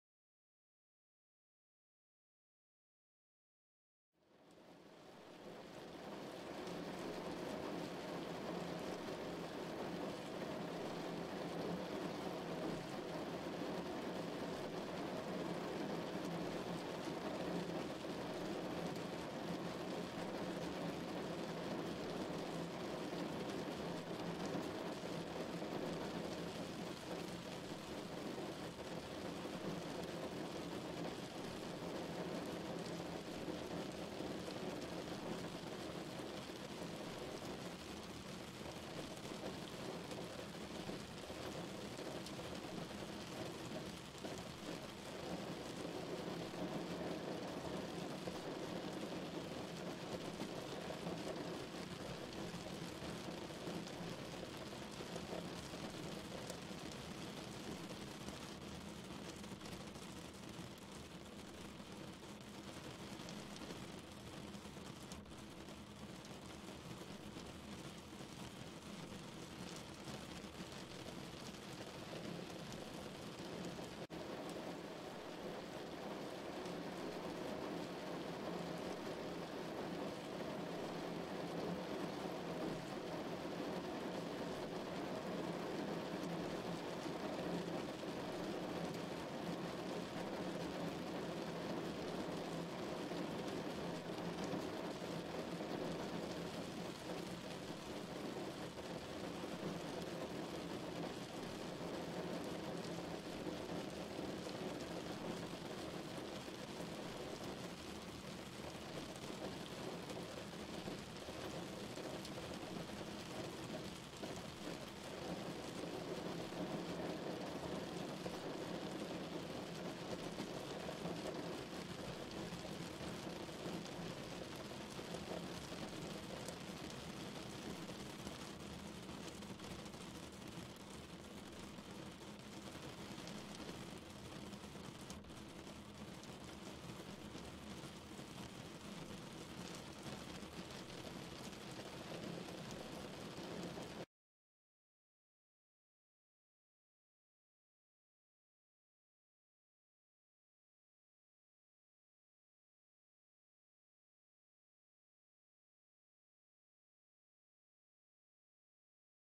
3. Relaxation - La pluie …
Le bruit de la pluie est un grand classique de relaxation.

La sensation de bien-être procuré par le son de la pluie sur un toit vient du sentiment de sécurité induit par le fait d’être à l’abri de celle-ci et bien au chaud.